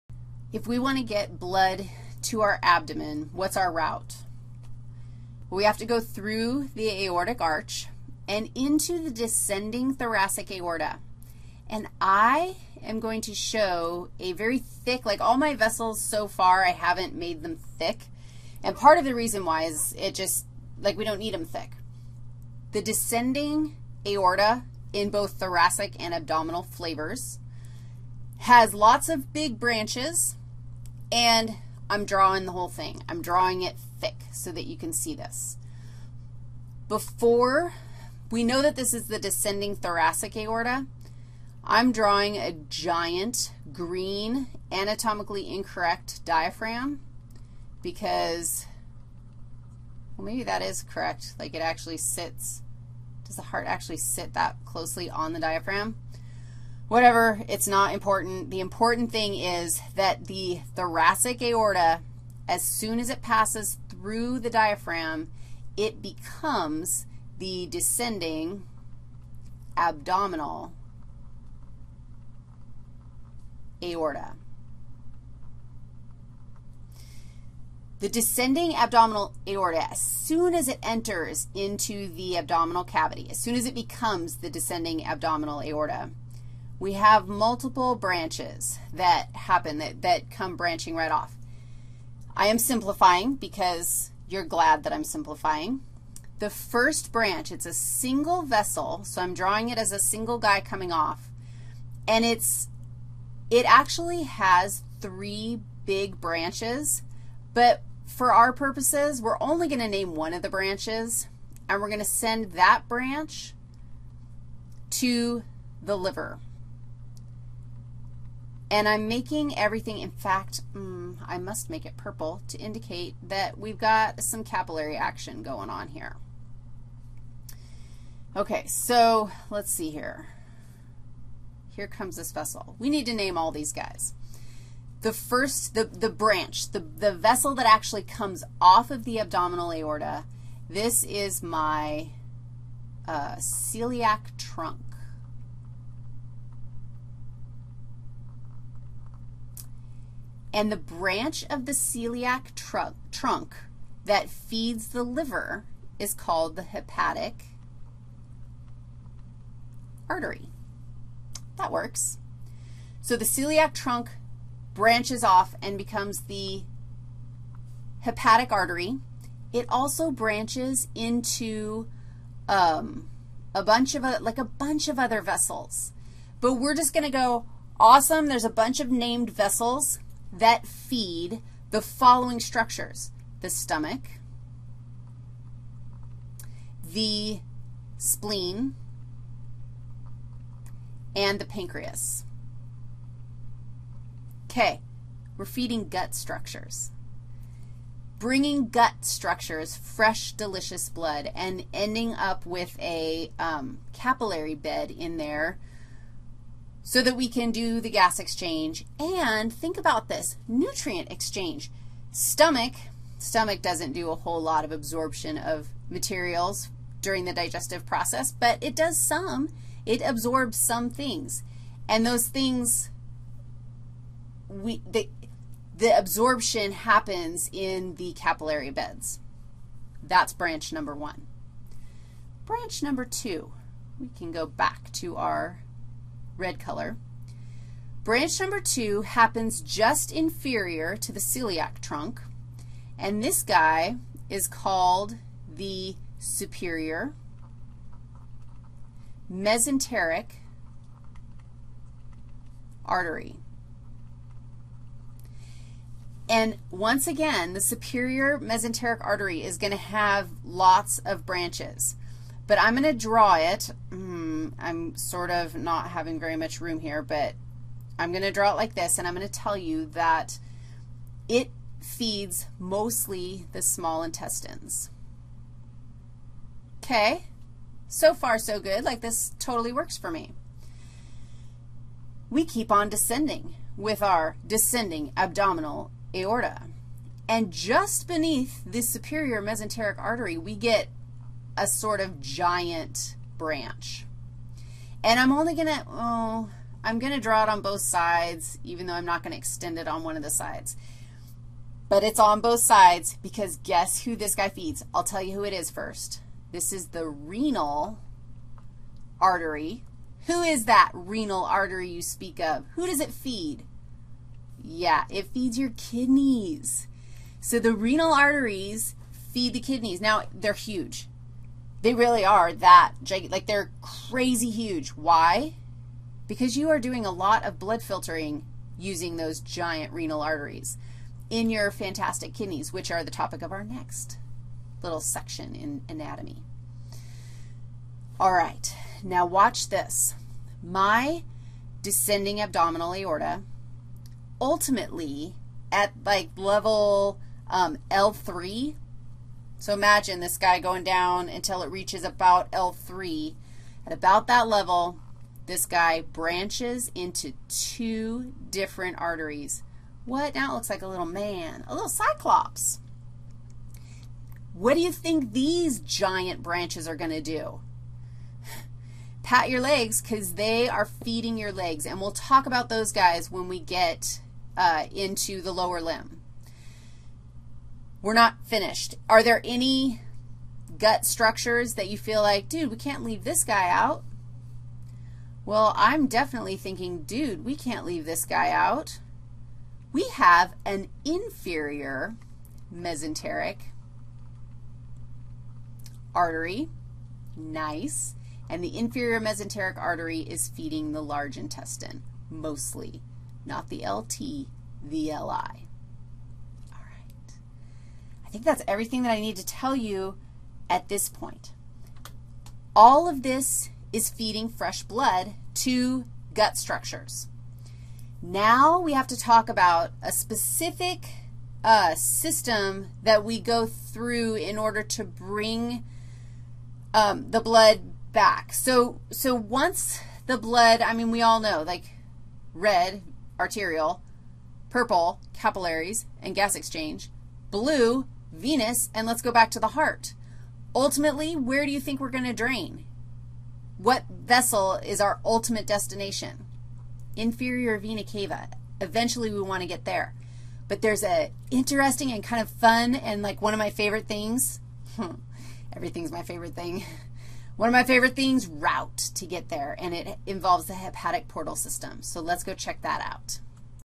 Circulation 4- Abdomen
Part 4 of 6 in a lecture on PERIPHERAL CIRCULATION in a flipped Human Anatomy course taught by Wendy Riggs. CC-BY. Watch the whole lecture (all 6 videos) by going to the PLAYLIST